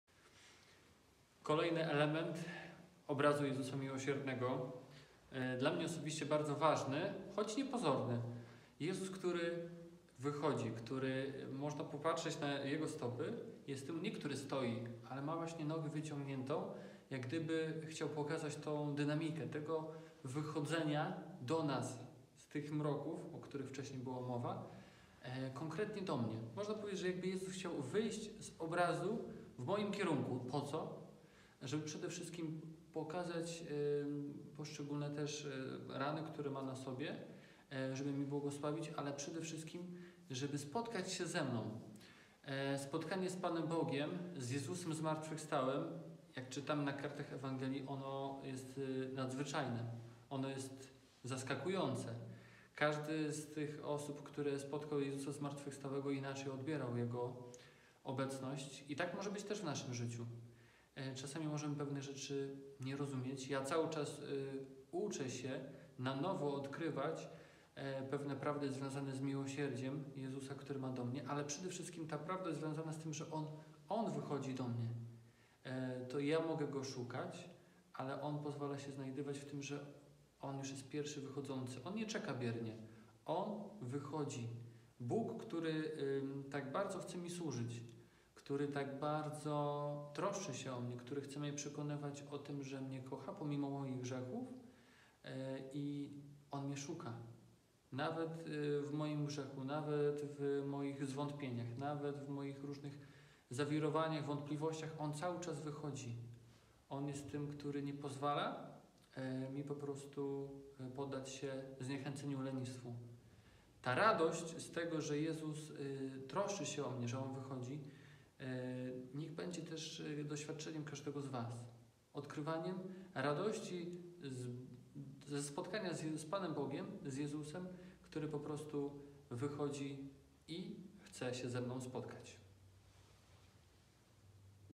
Spotkanko z Miłosiernym - [#3] Przychodzący
Trzecia katecheza przygotowująca na Niedzielę Miłosierdzia Bożego.
Ks. Wiesław ukazuje Miłosiernego, który zawsze wychodzi nam na spotkanie.